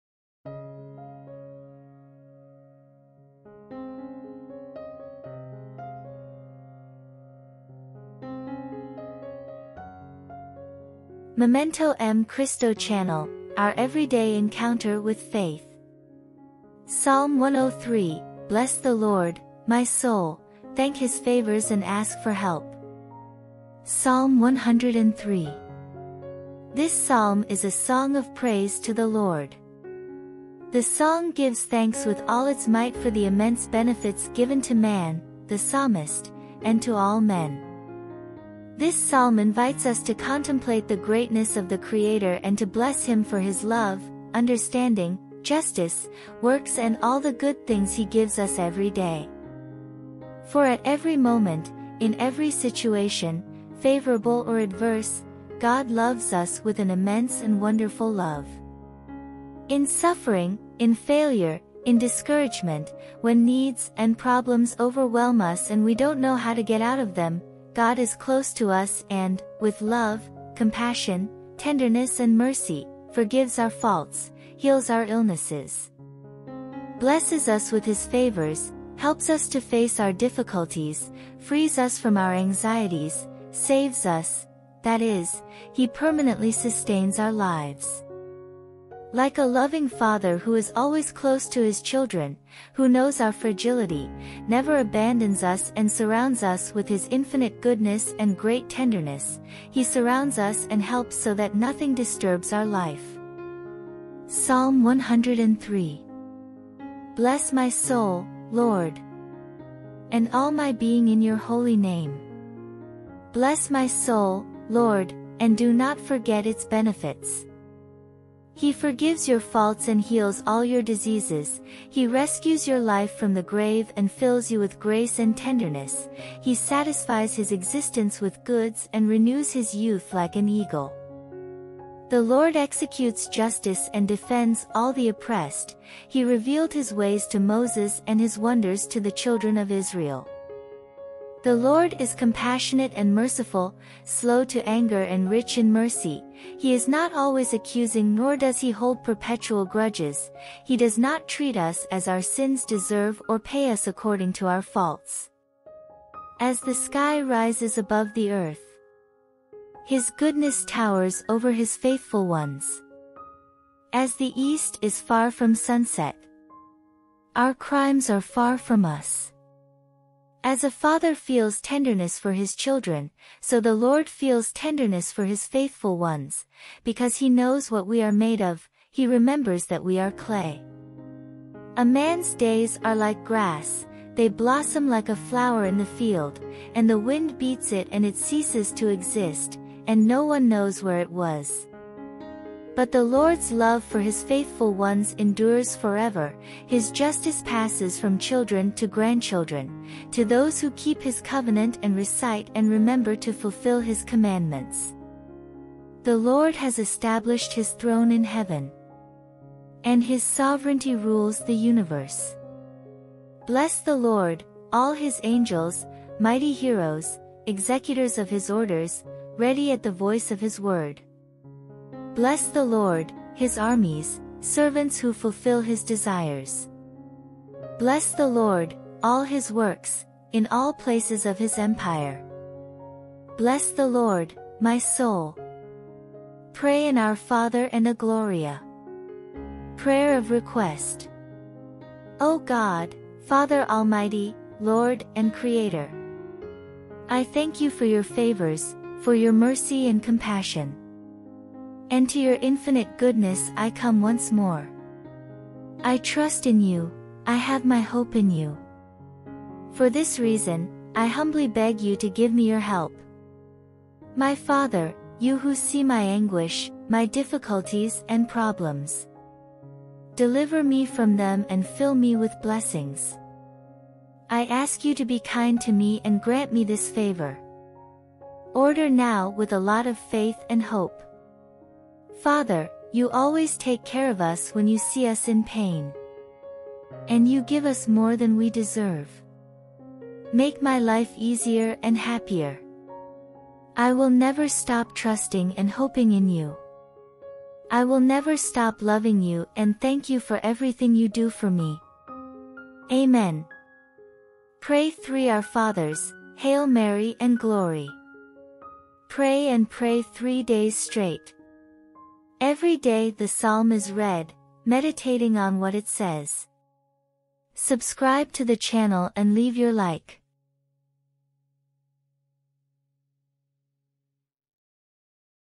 PSALM 103, BLESS THE LORD, MY SOUL 🙏🙏
New Videos - Monday to Sunday 06:30 and 18:30

PSALM 103, BLESS THE LORD, MY SOUL, (THANK HIS FAVORS AND ASK FOR HELP)

PSALM 103

This psalm is a song of praise to the Lord. The song gives thanks with all its might for the immense benefits given to man (the psalmist) and to all men.
This psalm invites us to contemplate the greatness of the Creator and to bless him for his love, understanding, justice, works and all the good things he gives us every day.

For at every moment, in every situation, favorable or adverse, God loves us with an immense and wonderful love. In suffering, in failure, in discouragement, when needs and problems overwhelm us and we don't know how to get out of them, God is close to us and, with love, compassion, tenderness and mercy, forgives our faults, heals our illnesses. , blesses us with his favours, helps us to face our difficulties, frees us from our anxieties, saves us... that is, he permanently sustains our lives.
like a loving Father who is always close to his children, who knows our fragility, never abandons us and surrounds us with his infinite goodness and great tenderness, he surrounds us and helps so that nothing disturbs our life.

PSALM 103

"Bless my soul, Lord,
and all my being in your Holy Name.

Bless my soul, Lord, and do not forget its benefits. He forgives your faults and heals all your diseases; He rescues your life from the grave and fills you with grace and tenderness; He satisfies his existence with goods and renews his youth like an eagle. The Lord executes justice and defends all the oppressed; He revealed his ways to Moses and his wonders to the children of Israel. The Lord is compassionate and merciful, slow to anger and rich in mercy; he is not always accusing nor does he hold perpetual grudges; He does not treat us as our sins deserve or pay us according to our faults.

As the sky rises above the earth,
his goodness towers over his faithful ones;
As the east is far from sunset,
our crimes are far from us. As a father feels tenderness for his children, so the Lord feels tenderness for his faithful ones; Because He knows what we are made of, He remembers that we are clay. A man's days are like grass, they blossom like a flower in the field, and the wind beats it and it ceases to exist, and no one knows where it was. But the Lord's love for his faithful ones endures forever, his justice passes from children to grandchildren: to those who keep his covenant and recite and remember to fulfill his commandments.

The Lord has established his Throne in Heaven,
and his sovereignty rules the universe. Bless the Lord, all his Angels, mighty heroes, executors of his orders, ready at the voice of his Word. Bless the Lord, his armies, servants who fulfill his desires. Bless the Lord, all his works, in all places of his empire. Bless the Lord, my soul!" Pray an Our Father and a Gloria. PRAYER OF REQUEST

O God, Father Almighty, Lord and Creator,
I thank you for your favors, for your mercy and compassion
and to your infinite goodness I come once more,
I trust in You, I have my hope in You,
For this reason, I humbly beg you to give me your help.

My Father, You who see my anguish, my difficulties and problems,
deliver me from them and fill me with blessings,
I ask you to be kind to me and grant me this favor:

Father, you always take care of us when you see us in pain.
and you give us more than we deserve,
make my life easier and happier,
I will never stop trusting and hoping in you,
I will never stop loving you and thank you for everything you do for me.
Amen .

Pray three Our Fathers, Hail Mary and Glory.

Pray and pray three days straight,
(Every day the Psalm is read, meditating on what it says).